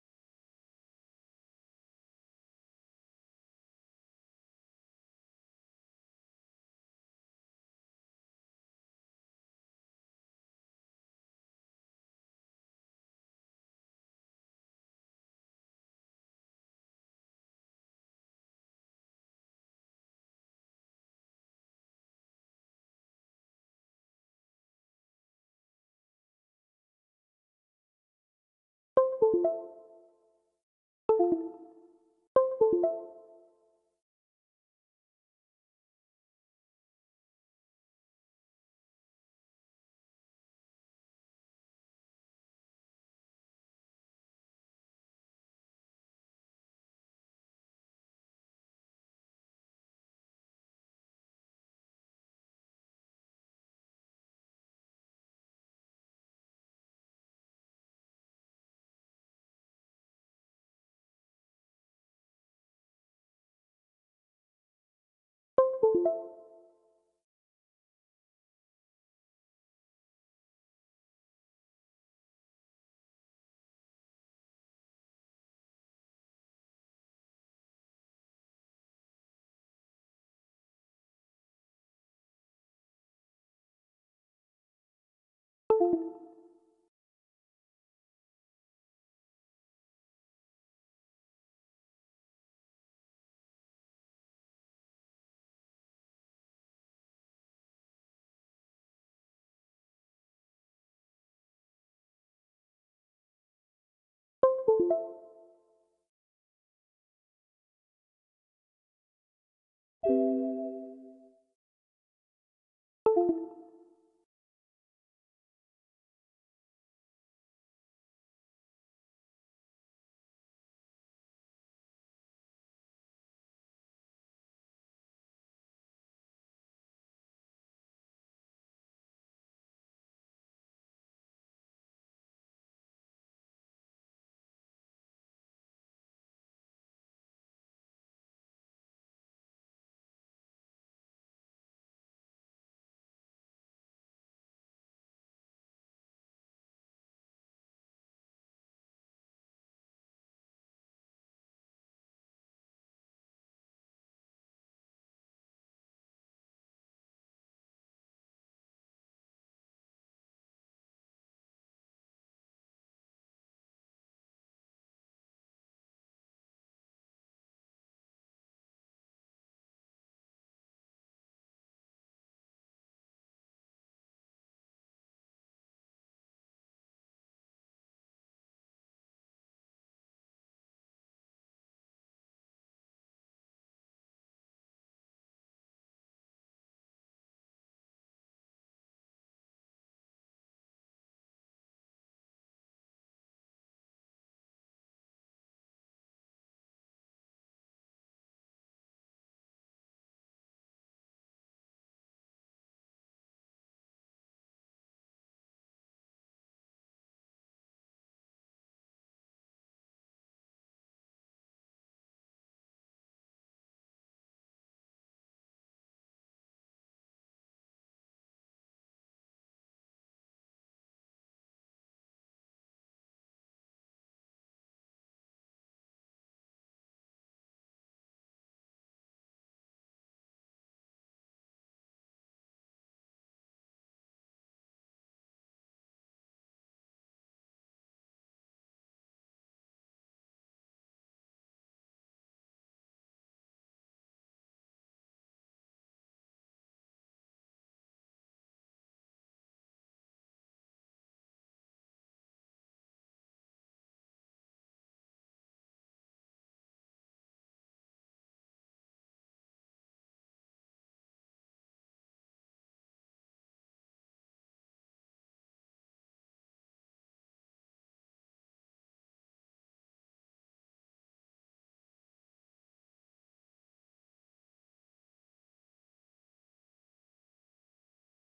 Bypass Google Account Intex Aqua Wing FRP
How to bypass Google Account / Gmail Account (FRP) in Intex  Aqua Wing ?